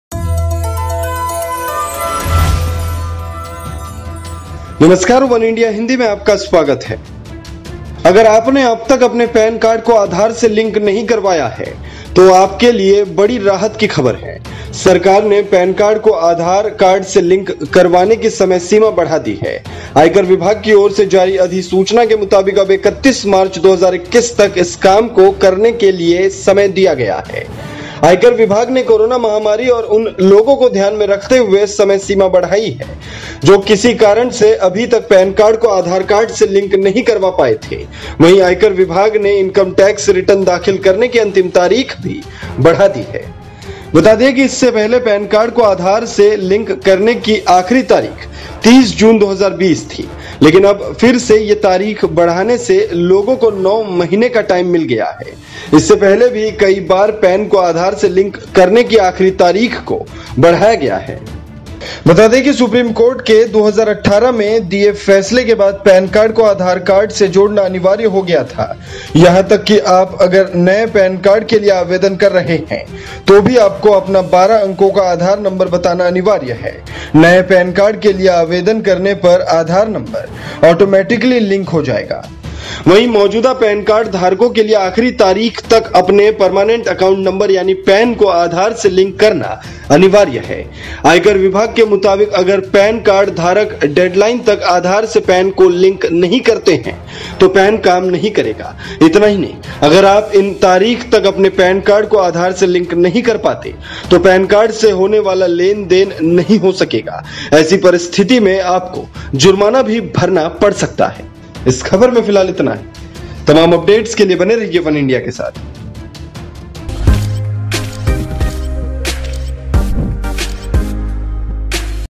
Aadhar PAN Link: सरकार ने PAN और Aadhaar को लिंक करने की आखिरी तारीख बढ़ाई | वनइंडिया हिंदी
The Union Finance Ministry has extended the time limit for linking PAN card to Aadhaar card. According to the notification issued by the ministry, now time has been given to do this work till 31 March 2021. Even before this date has been extended many times. The ministry has extended the time limit keeping in mind the corona epidemic and those people who for some reason were not yet able to link the PAN card to the Aadhaar card.

अगर आपने अब तक अपने पैन कार्ड को आधार से लिंक नहीं करवाया है तो आपके लिए बड़ी राहत की खबर है।  केंद्रीय वित्त मंत्रालय ने पैन कार्ड को आधार कार्ड से लिंक करवाने की समय सीमा बढ़ा दी है. मंत्रालय की ओर से जारी अधिसूचना के मुताबिक अब 31 मार्च 2021 तक इस काम को करने के लिए समय दिया गया है.